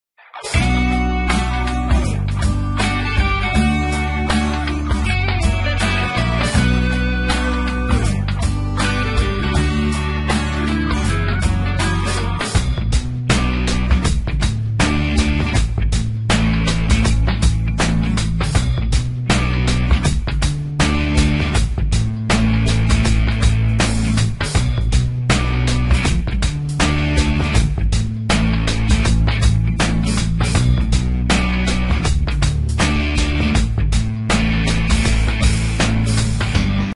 An obedient puppy#Short video
It was a cute and obedient little dog. He asked it to take something and really brought it here.